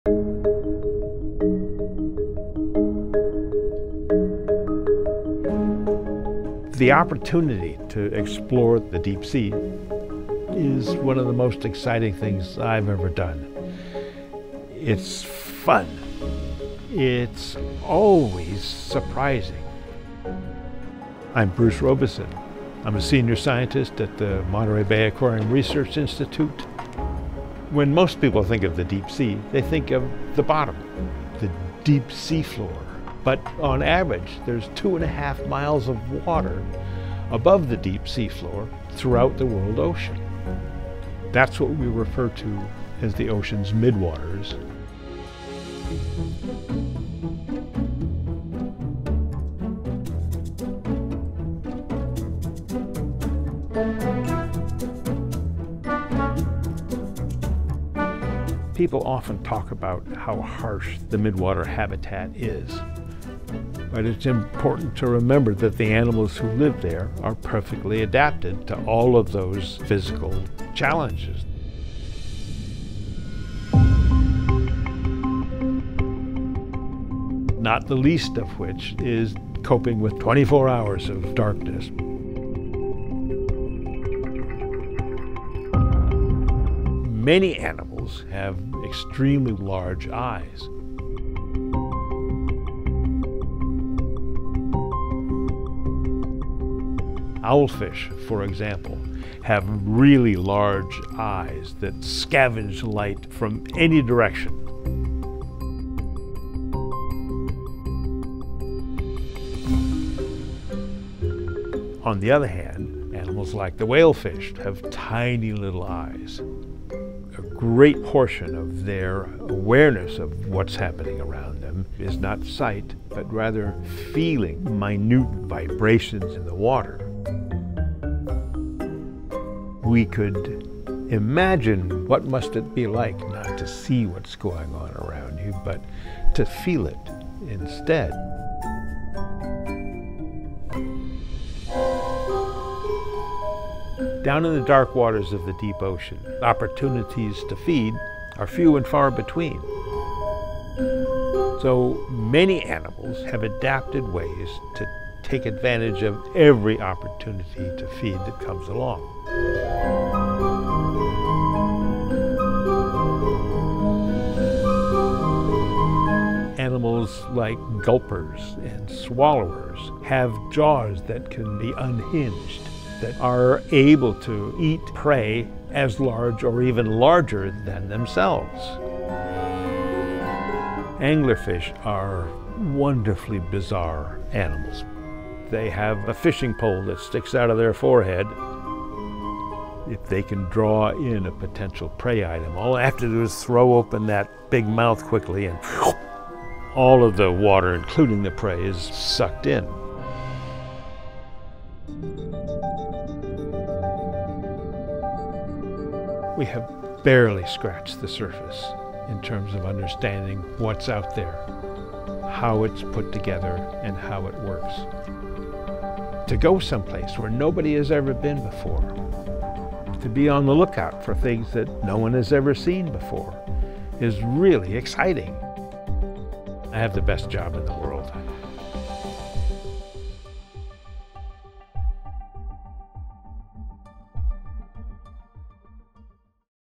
What creatures live in the "Midnight Zone" of the deep sea?
Join @MBARIvideo's Senior Scientist Bruce Robison on a dive into the vast midwater of the deep sea—the Midnight Zone!

This largest living space on Earth lies between our ocean’s sunlit upper layers and the deep seafloor, and Bruce has studied and explored the midnight zone for his entire career. The stories he tells about the discoveries MBARI has  made boggle the mind, inspiring awe and wonder about how animals have adapted to this dark, cold, fluid world. Most of these animals don’t look or act like their shallow-water relatives, and have evolved an incredible array of strategies to not only survive in the midnight zone, but thrive there.

The midnight zone is home to our planet’s largest ecosystems, and the life that thrives there likely outnumbers all other animals on the planet. And yet, we are only beginning to understand this vast ecosystem with each research expedition.

Stay tuned for more from the Monterey Bay Aquarium and our colleagues at MBARI as we prepare for the launch of the 2022 exhibit, "Into the Deep: Exploring Our Undiscovered Ocean."

What kind of creatures live in the "Midnight Zone" of the deep sea? | Into The Deep